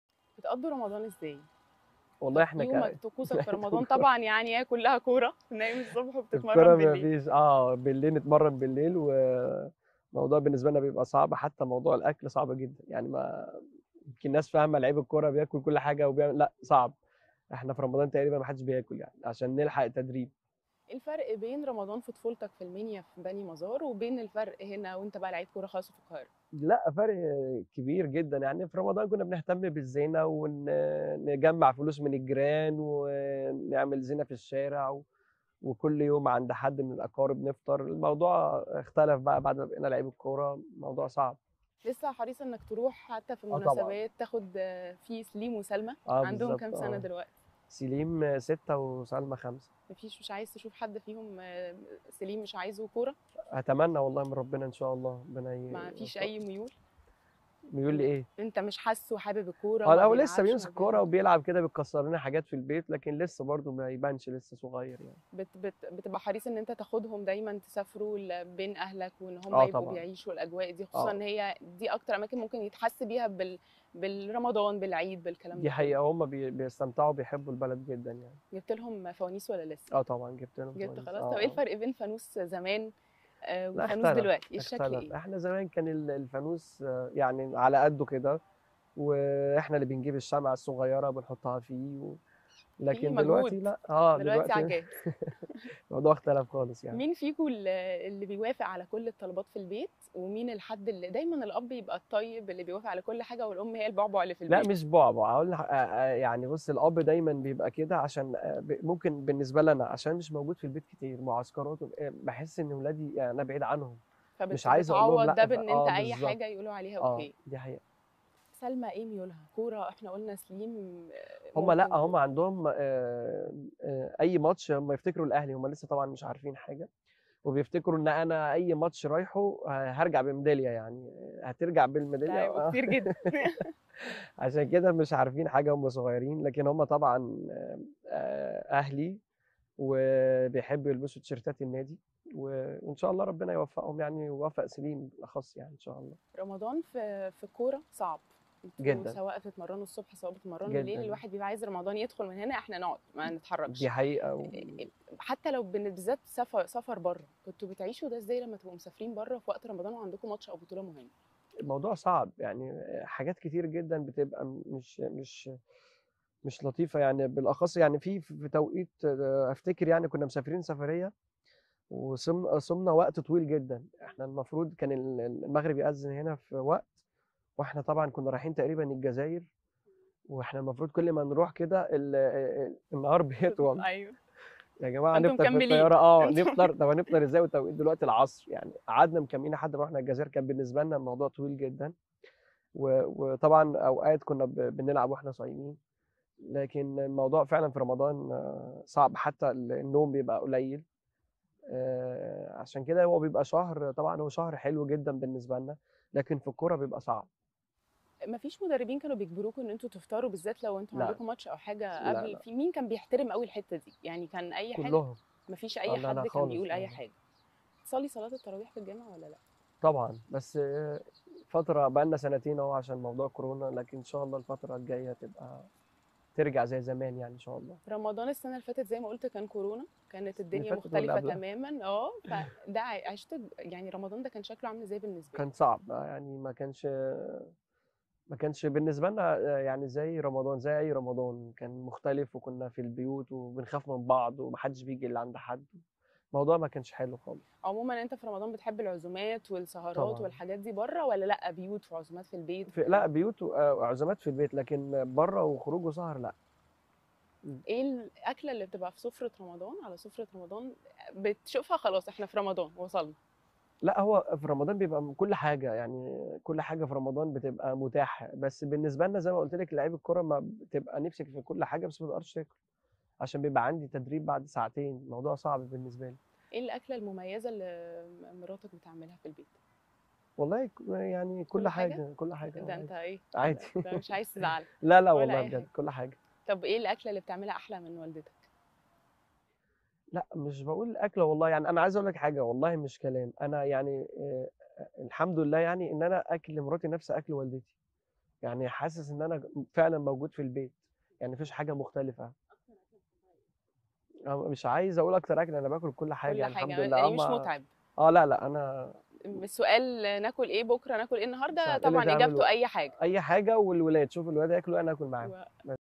الكابتن وليد سليمان يتحدث عن طقوس اسرته في شهر رمضان المبارك.. ويكشف كواليس المعسكرات في رمضان.
الكابتن وليد سليمان يتحدث عن طقوس اسرته في شهر رمضان المبارك.. ويكشف كواليس المعسكرات في رمضان | لقاء خاص